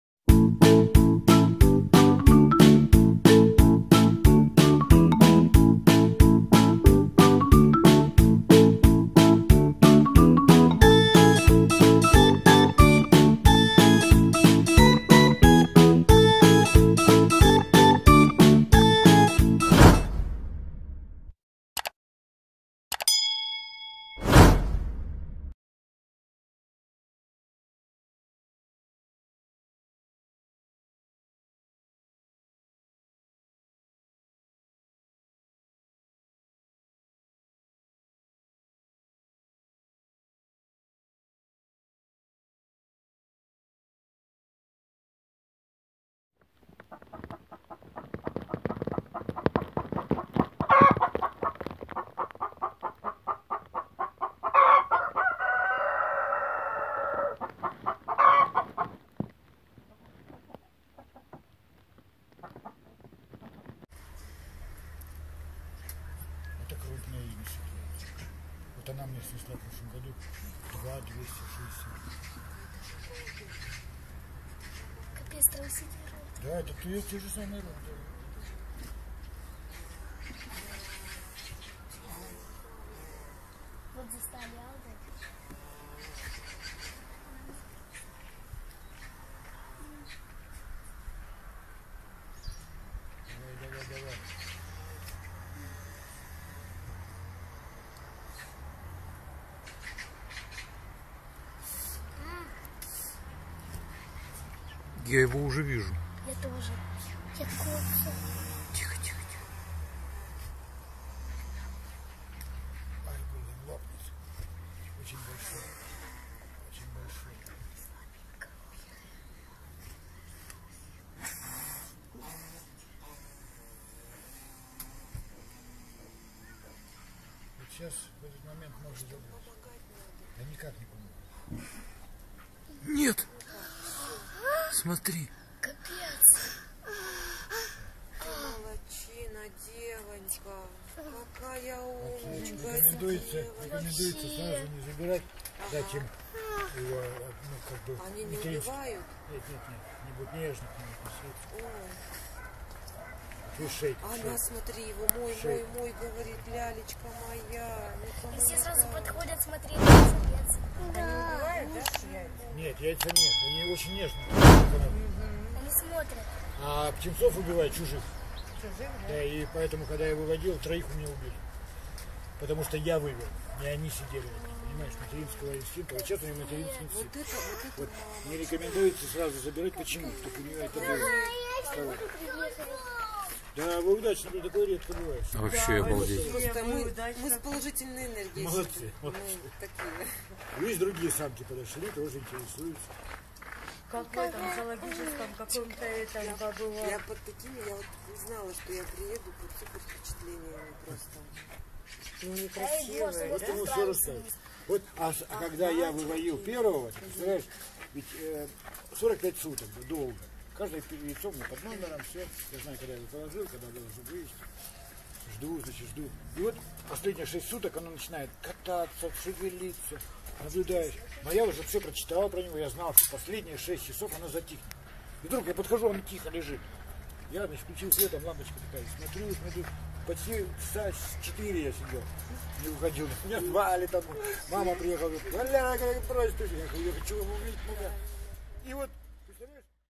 BIRDS LAYING EGGS || Close-up..!! BIG and SMALL BIRDS laying EGGS...
If you see a clip that you own that you did not submit or give consent for use, we have likely received a false permissions and would be happy to resolve this for you!